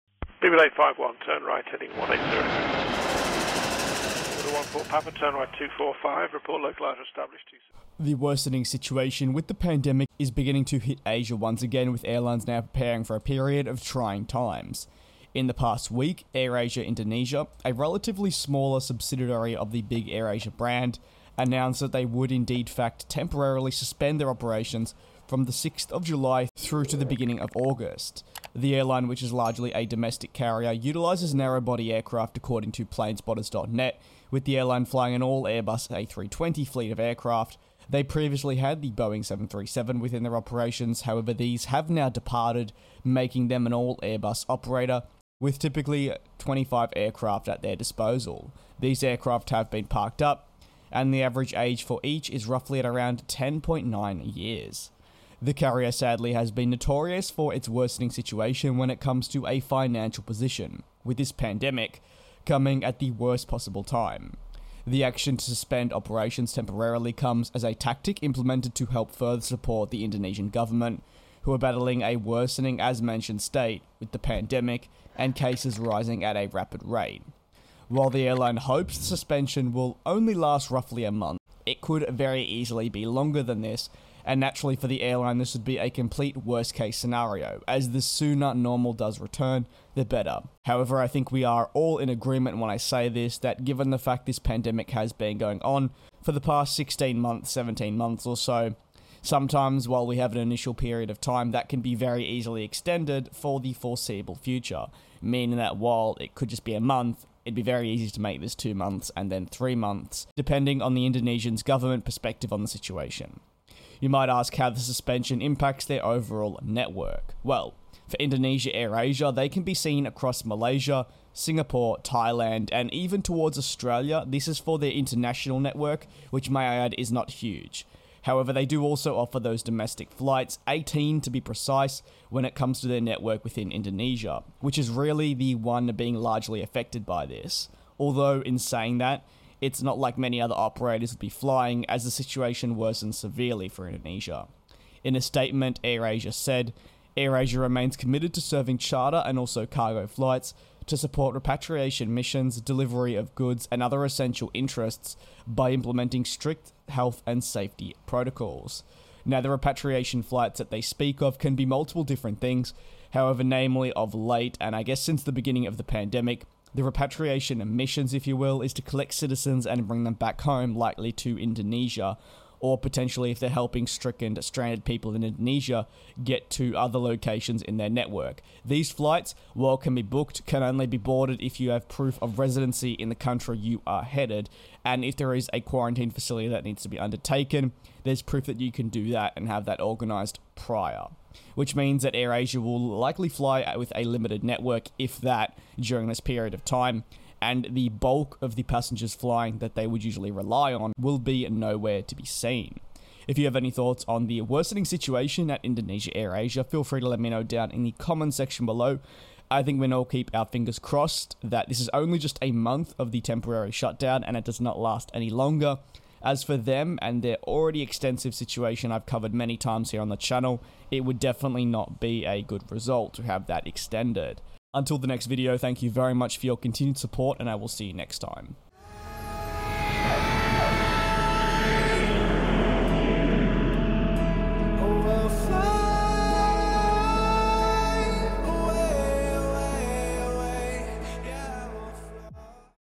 Sad Indonesia Air Asia News
The Situation at Indonesia Air Asia is worsening following sad developments coming out of Indonesia with regards to the ongoing global pandemic which is now subsequently impacting the airlines overall route network. For an already struggling carrier, the developments hit them 10x hard. In today's video, I take a look at the news.